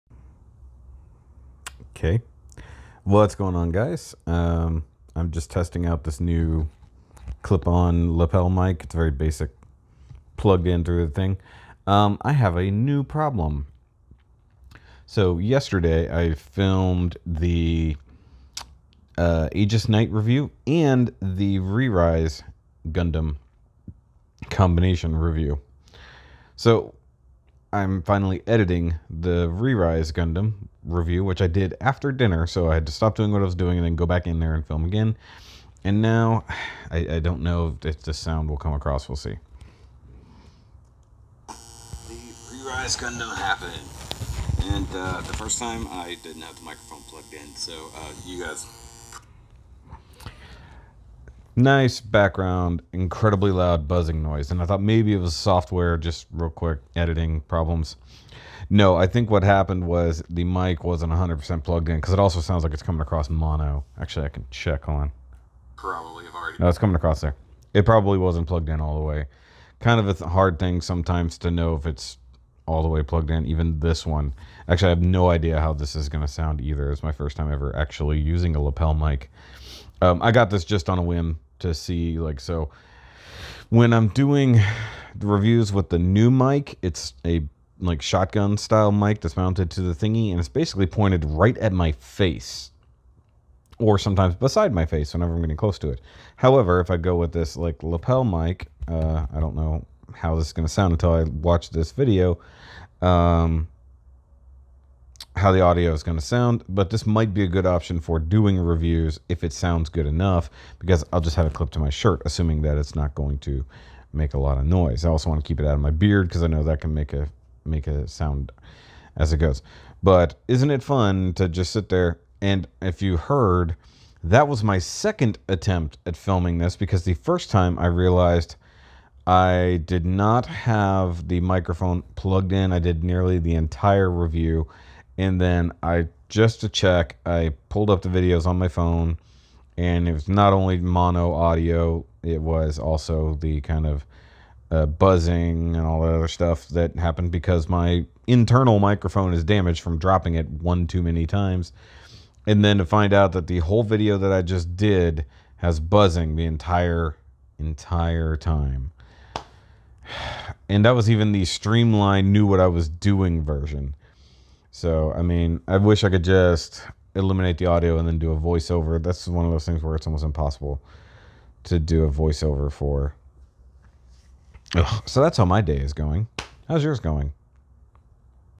THIS REVIEW IS CURSED
What is even happening? Having to record it TWICE. Then audio problems. AND THEN when I try a voice over that doesn't even go well.